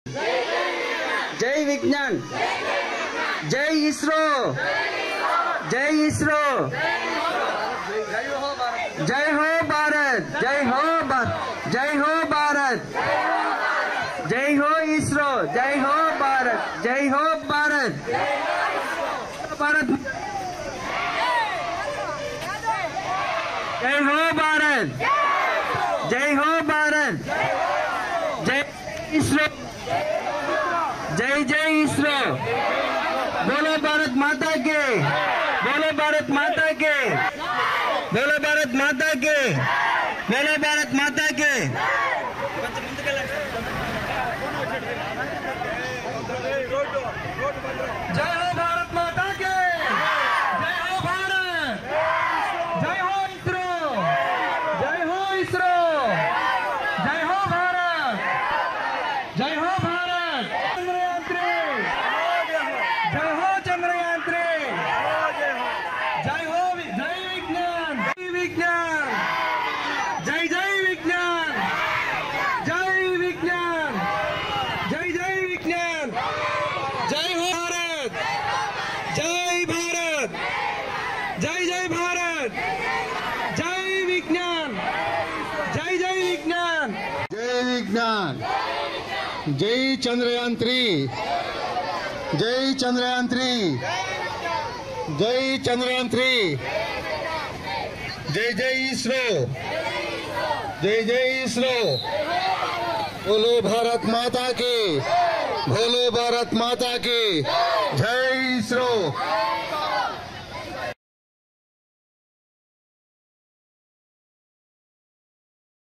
JAI HO CHANDRAYAAN 3 @My School
educational video
Chandrayaan-3 is the third Indian lunar exploration mission under the Indian Space Research Organisation's Chandrayaan programme. It consists of a lander named Vikram and a rover named Pragyan, similar to those of the Chandrayaan-2 mission.